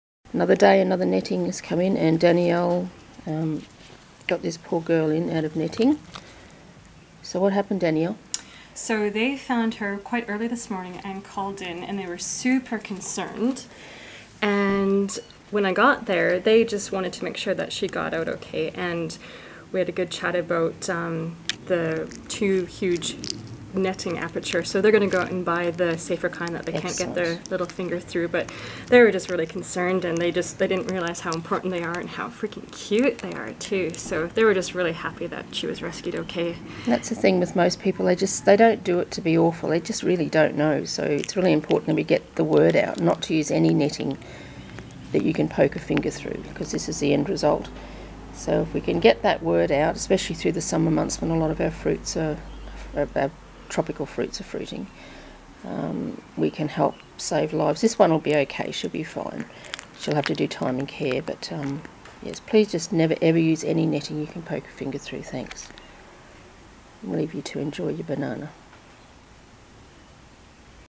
Ripe Banana Saves The Day
Rescued out of large aperture netting by Danielle this morning, Miss Roberta is now safely in care enjoying all the bananas she can eat!
The homeowners were more than happy to replace their netting with a wildlife safe version thus ensuring that no more flying-foxes will be caught.
Please never use any netting you can poke a finger through and instead choose shade cloth type netting.